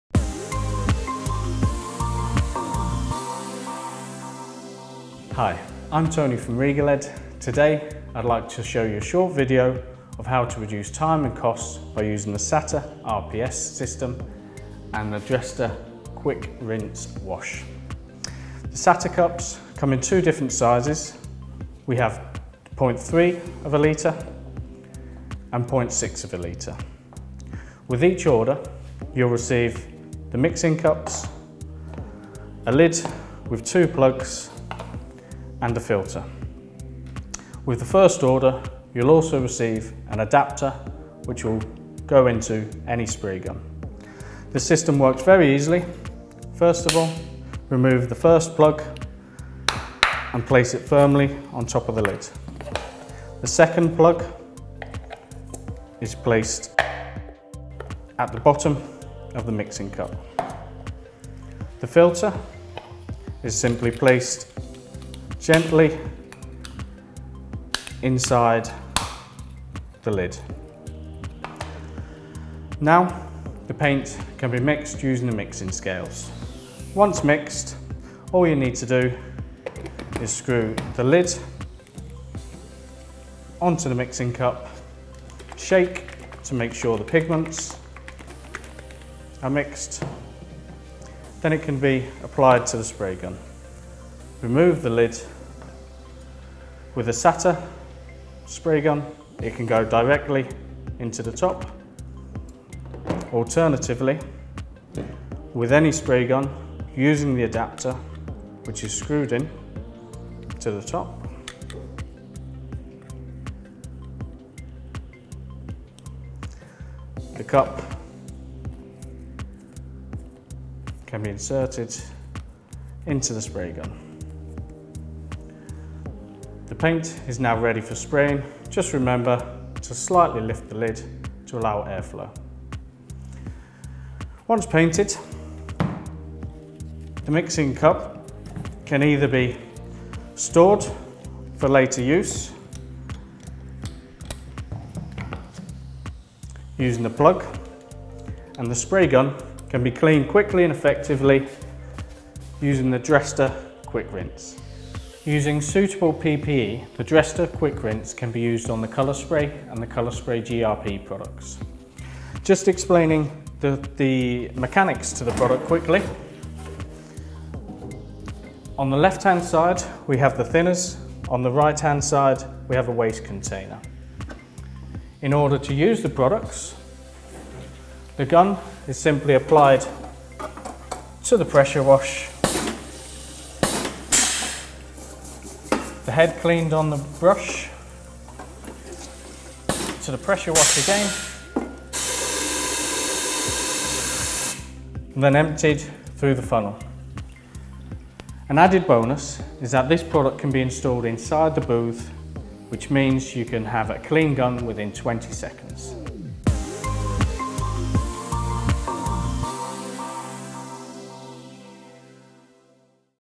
SATA Rapid Preparation System - Regalead tutorial
SATA (RPS) is the cup system for bodyshops emphasising perfect painting finishes and high efficiency.
SATA RPS facilitates the painters’ work, significantly enhances the bodyshop’ productivity and critically reduces the use of cleaning agents and solvents.